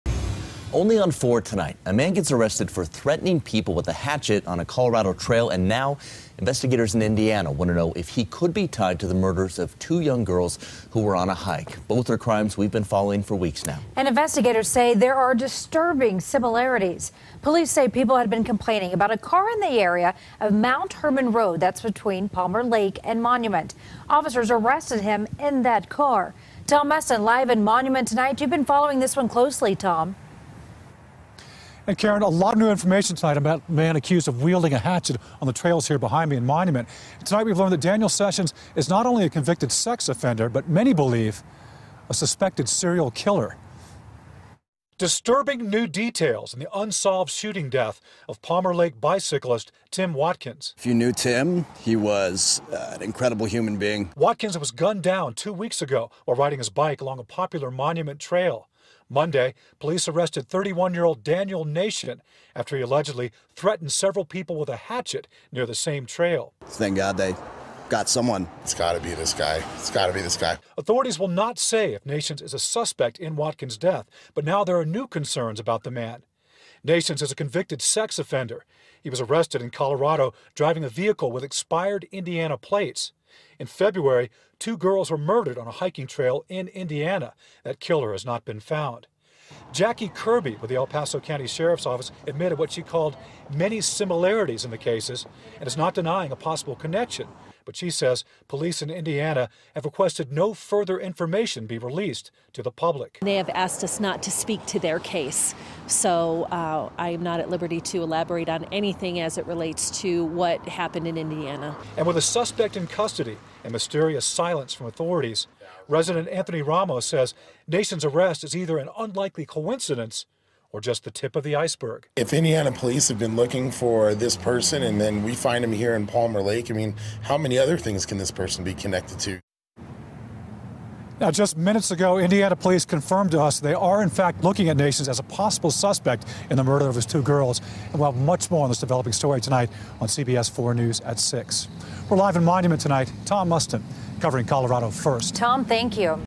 Suspected Hatchet Man May Be Linked To Murders In Indiana
Daniel Nations was arrested in connection with threatening cyclists.